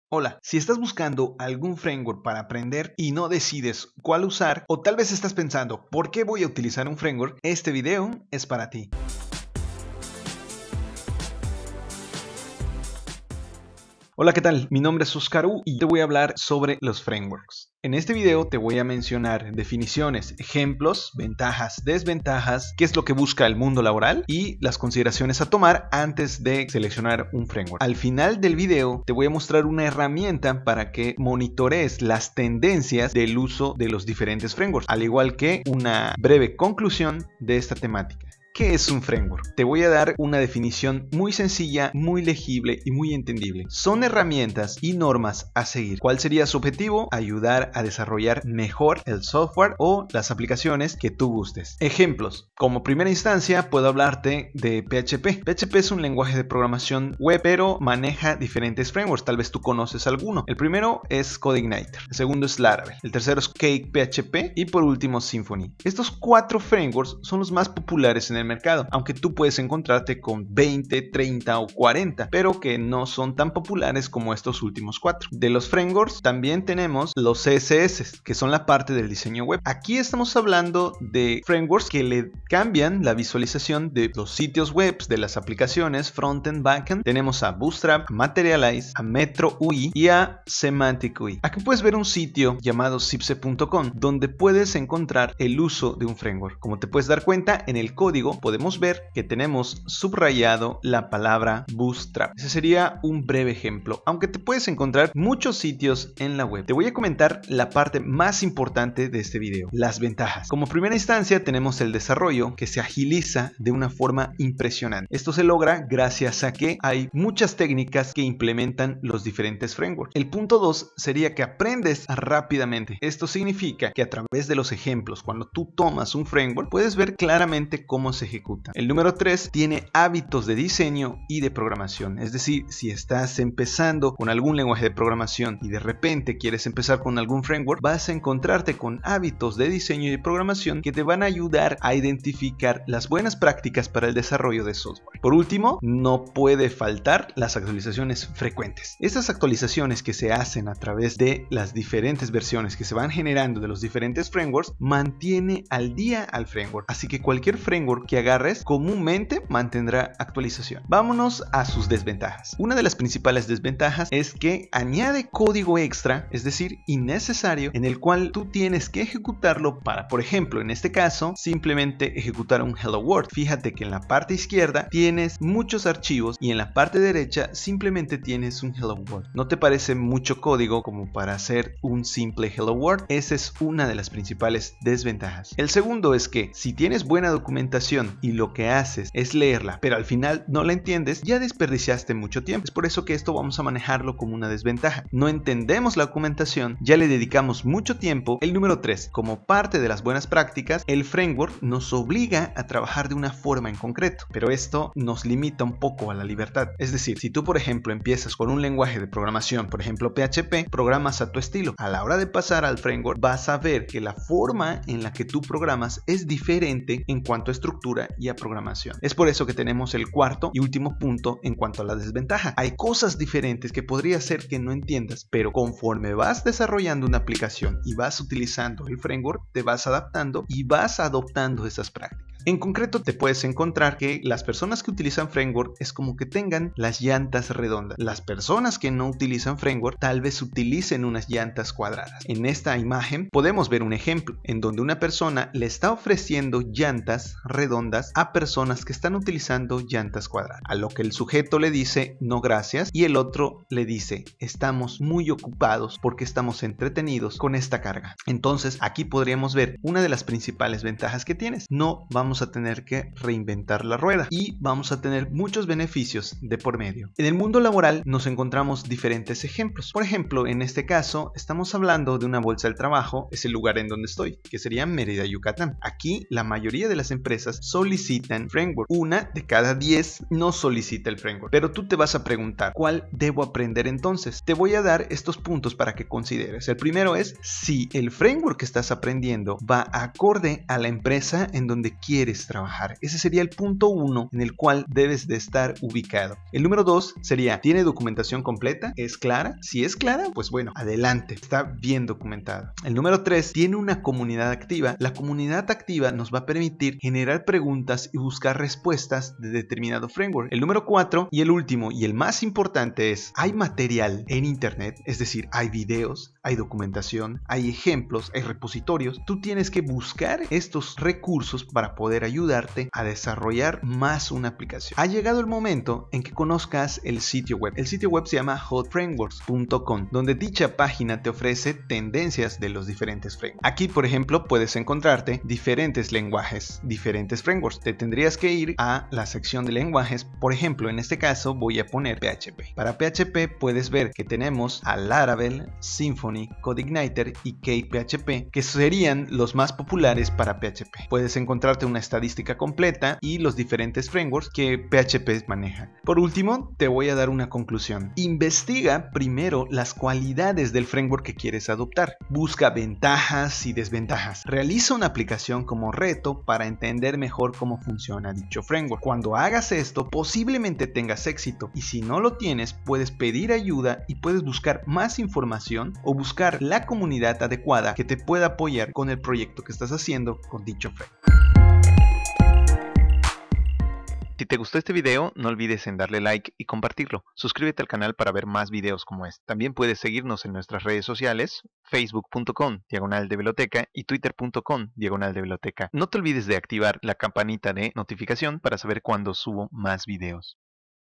Usar un Framework - ¿Por qué?
Este video es un video explicativo de lo que son los frameworks, ventajas, desventajas y herramientas que me definen que framework seleccionar.